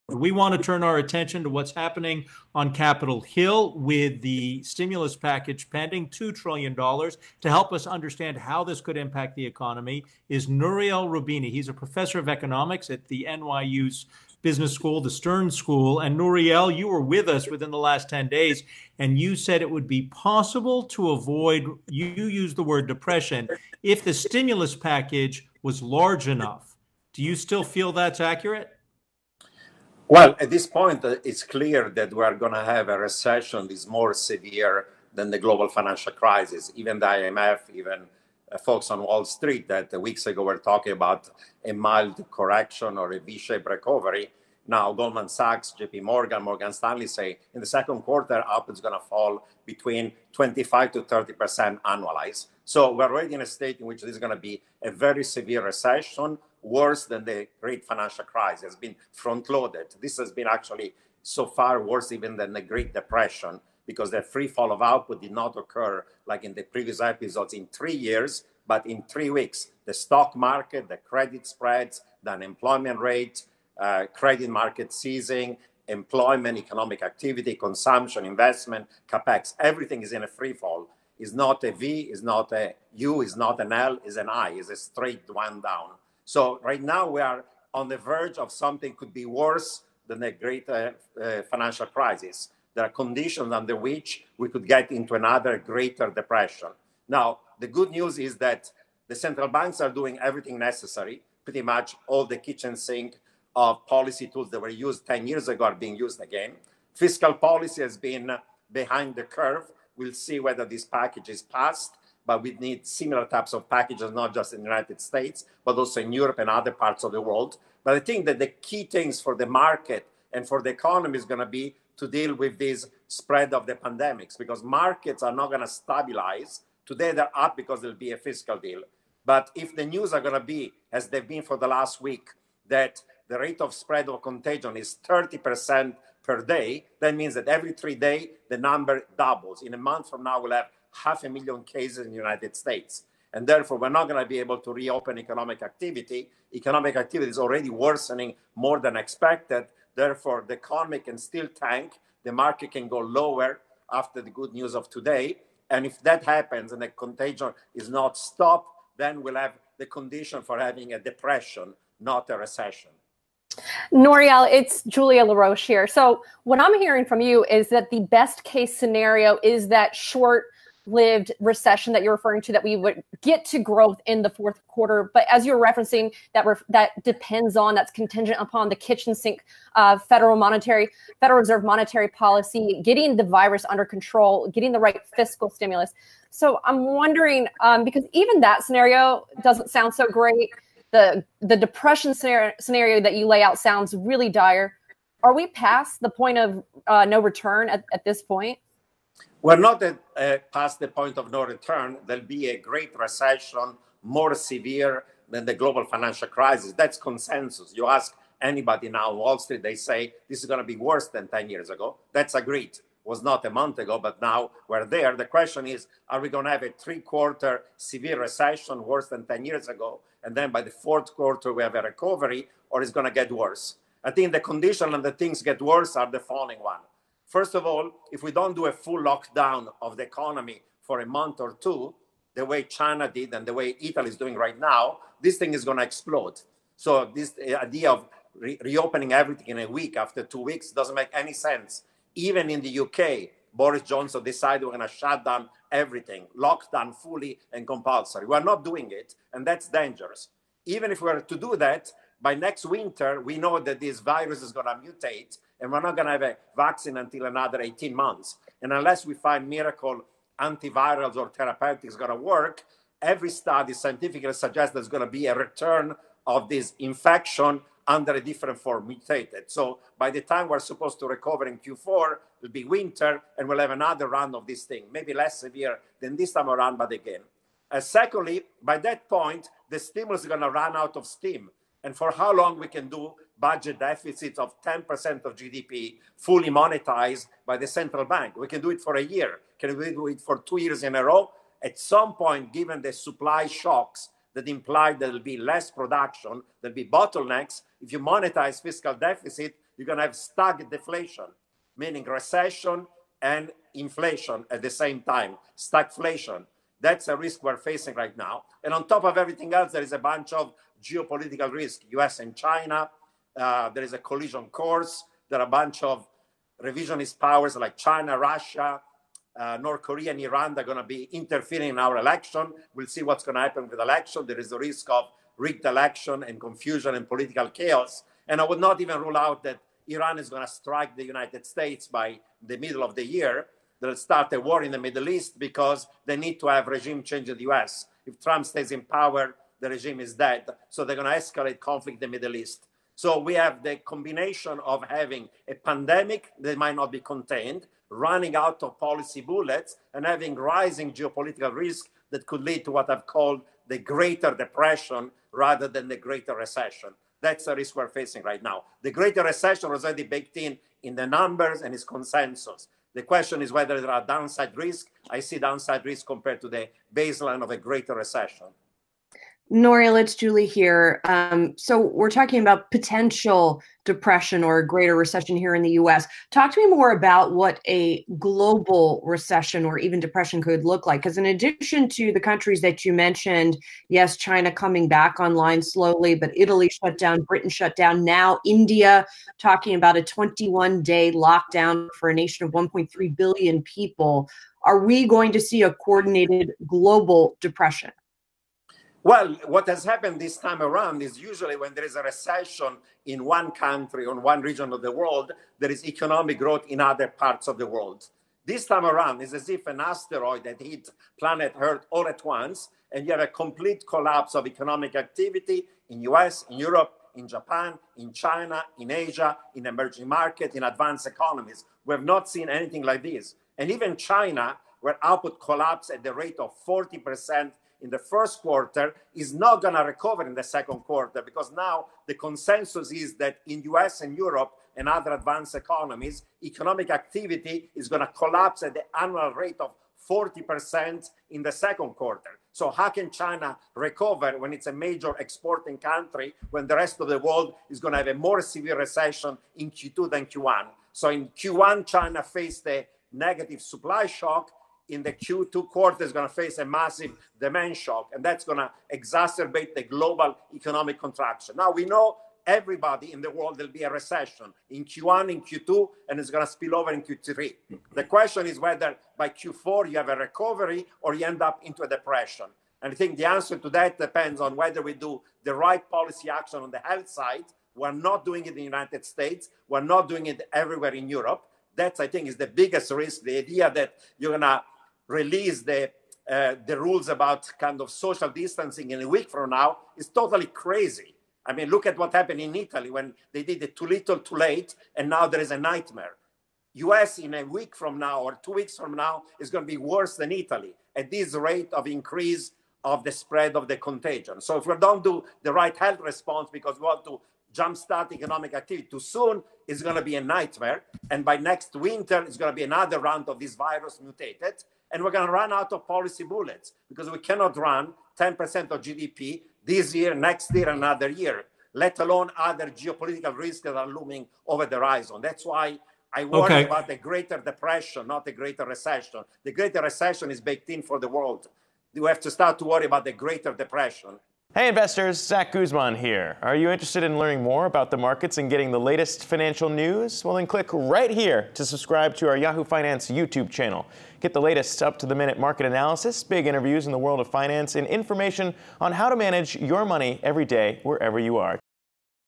Roubini: Coronavirus economic damage is 'worse even than the Great Depression'
Nouriel Roubini, Professor of Economics at NYU Stern, joins Yahoo Finance's On The Move panel to discuss how the government has handled the coronavirus pandemic and the severe economic damage coronavirus COVID-19 is having on the economy.